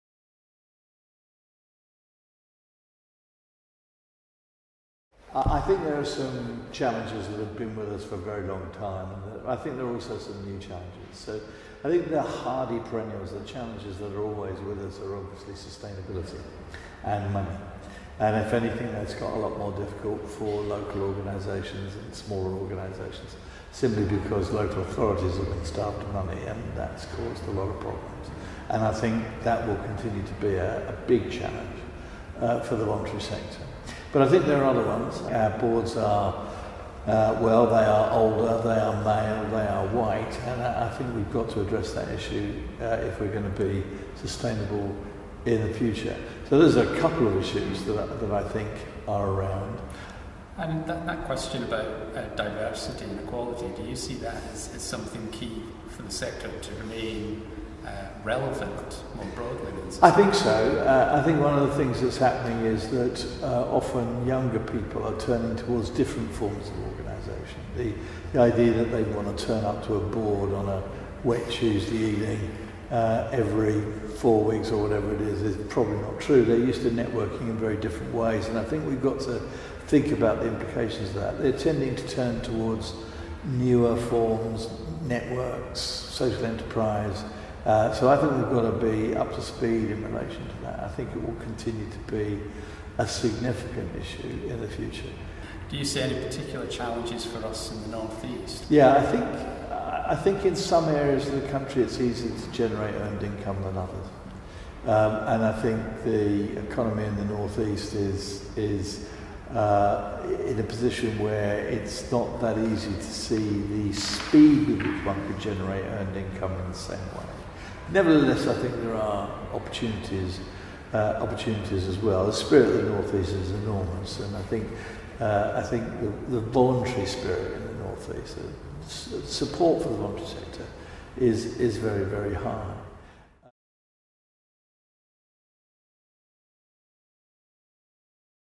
Sir Stuart Etherington on the challenges of the voluntary sector in the North East
Sir Stuart Etherington, Chief Executive of NCVO tells us about what he sees as the sustainability and funding challenges that face the future of our voluntary sector including the board of trustees; equality and diversity; newer forms of networking and being up to speed with it and the challenge of generating earned income in the North East.